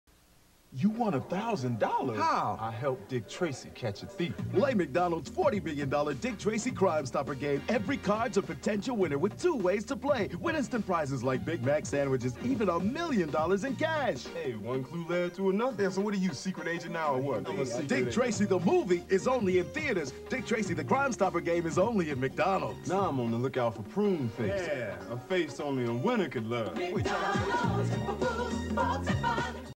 McDonald's Winning Faces Dick Tracy Crimestoppers game commercial 1990
Commercial from a June 12, 1990 recording of game 4 of the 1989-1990 NBA finals on WEYI TV 25 Saginaw CBS